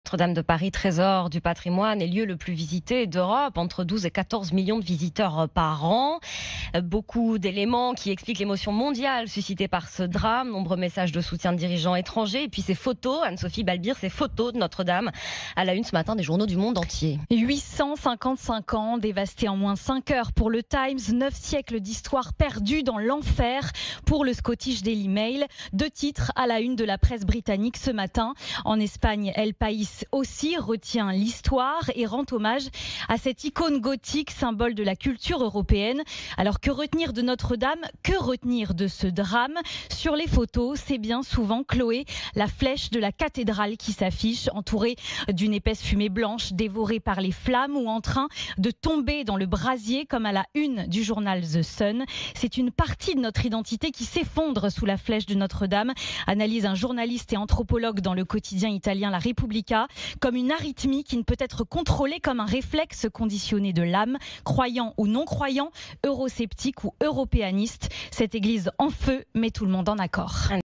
L'incendie de Notre-Dame de Paris à la une de la presse étrangère
Notre-Dame de Paris est un très important lieu de culte avec plus de 2000 offices chaque année et le lieu le plus visité d’Europe avec entre 12 et 14 millions de visiteurs par an et 30 000 par jour en moyenne. Autant d'éléments qui expliquent l'émotion mondiale suscitée par ce drame et les nombreux messages de soutien de dirigeants étrangers.